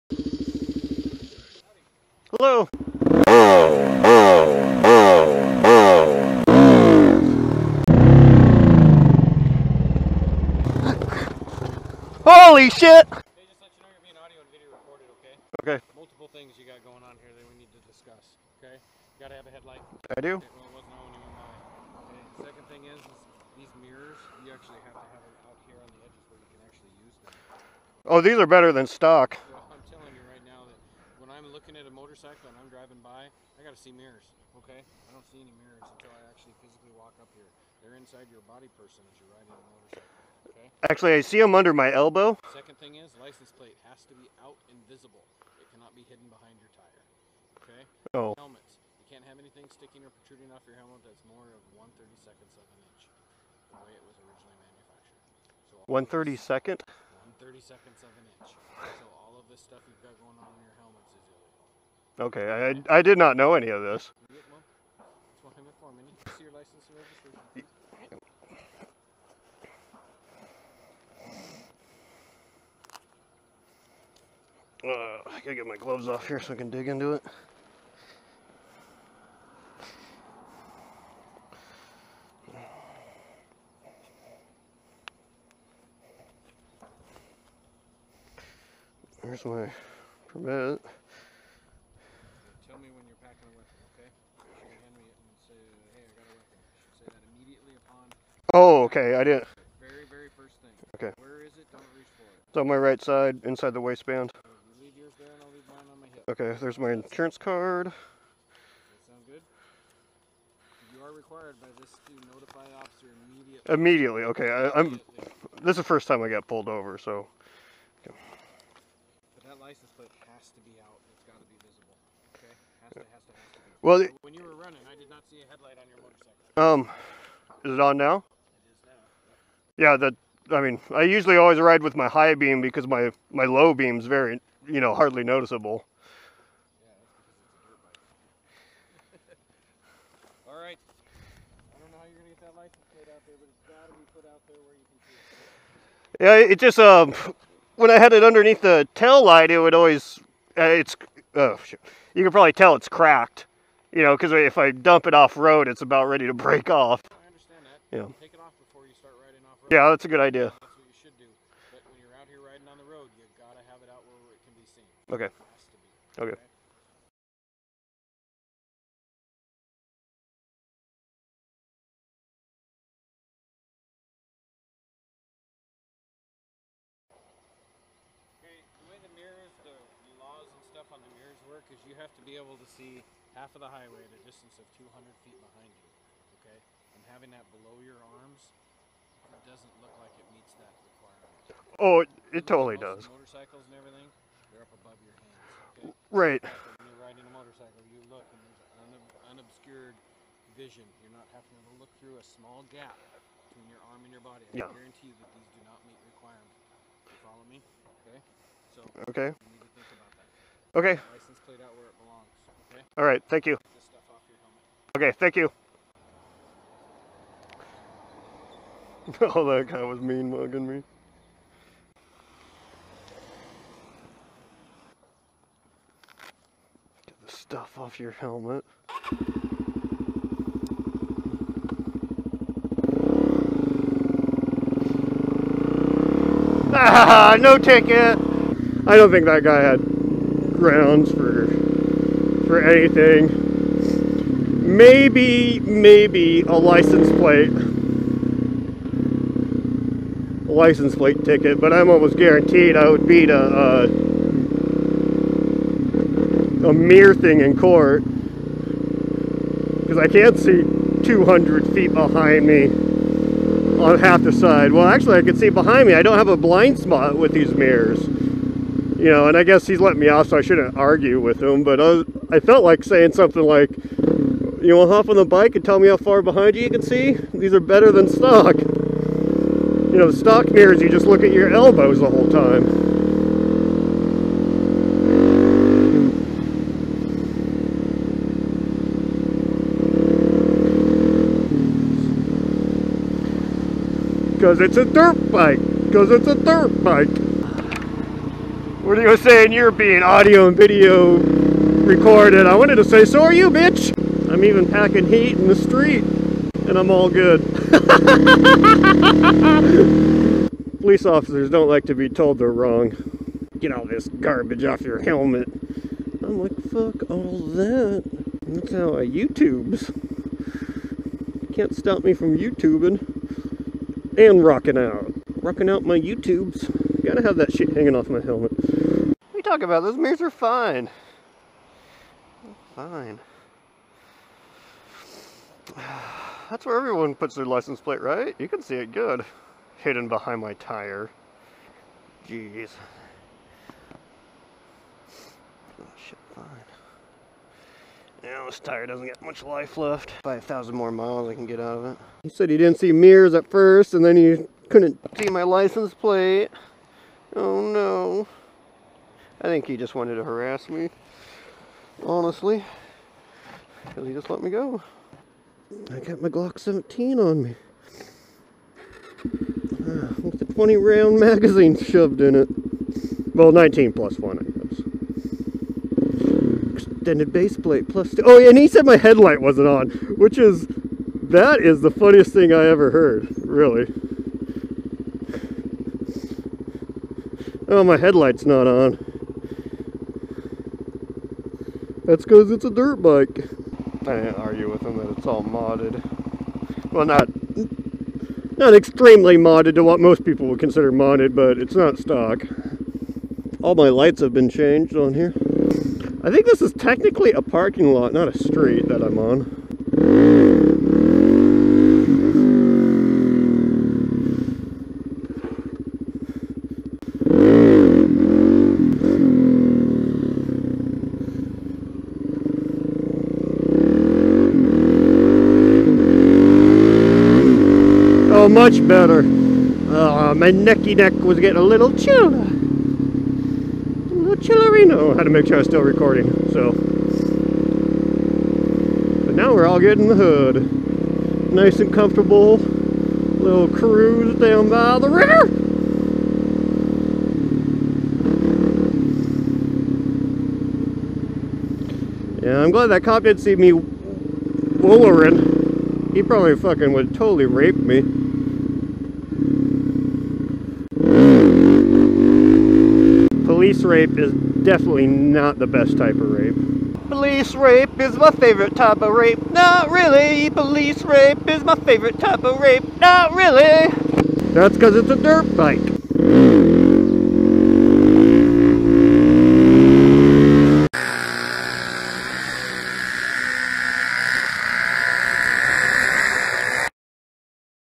Should I Run?!?
I'm carrying my Glock 17concealed when I get pulled over for no mirrors, no headlight,, hidden plate, and stuff prodruding from my helmet...